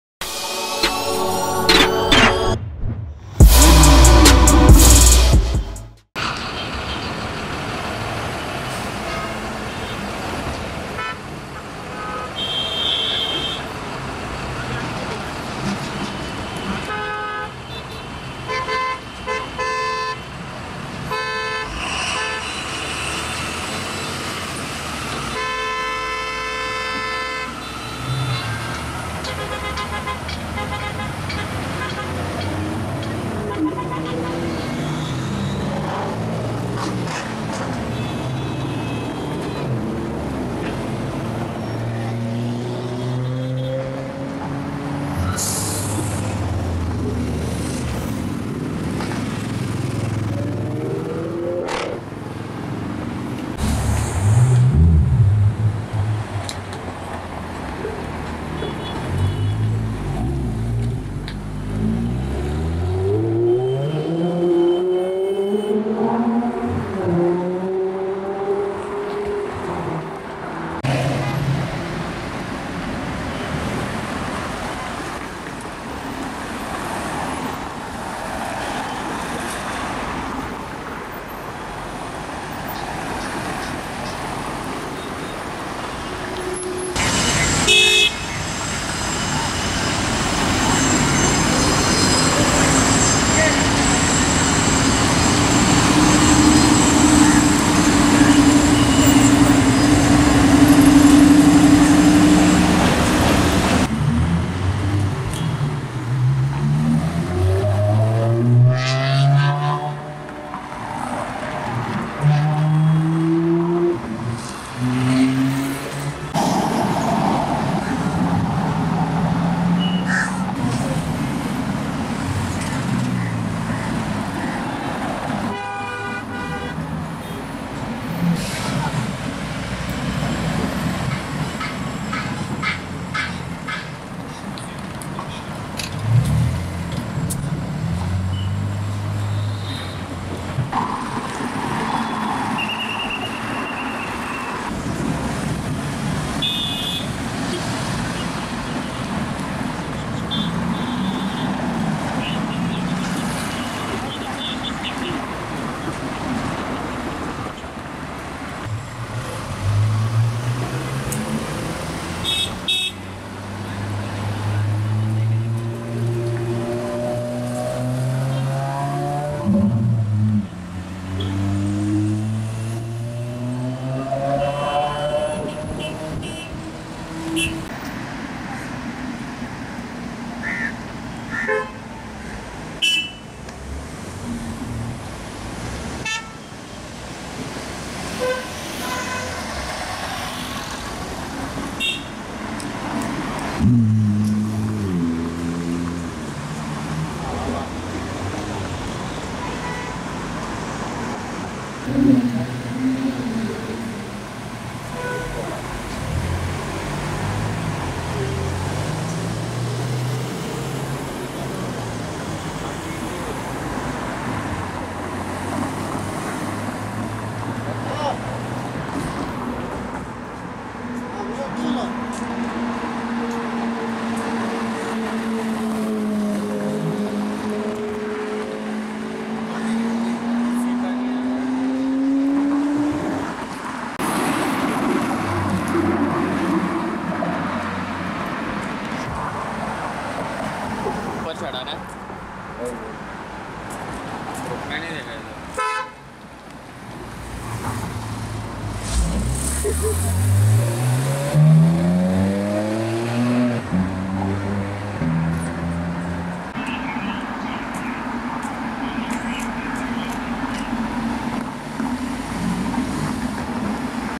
MADHURI DIXIT IN FERRARI 296 GTB & MANY SUPERCARS | SUPERCARS IN MUMBAI | INDIA | JANUARY 2023
We are back with another episode where you will find supercars, sportscar and exotics taking on the streets of mumbai.
Cars in this video are :
Ferrari 296 GTB w/ Madhuri dixit & Dr. Shriram Nene
Ferrari Portofino
Ferrari SF90 Stradale
Lamborghini Murcielago
Lamborghini Gallardo India Serie Speciale LP 550-2
Lamborghini Huracan RWD LP 580-2
Porsche 911 GT2RS 991
Porsche 911 GT3 991.1
Porsche 911 Carrera S 997.1
Bentley Continental Super Sports
BMW F90 M5
BMW M4
BMW M3
BMW M2
Nissan GTR R35 AMS Alpha 14
Ford Ranger
Ford Mustang GT
Mercedes Benz E63 AMG
Mercedes Benz CLA45 AMG

SUPERCARS IN INDIA | MUMBAI | JANUARY 2023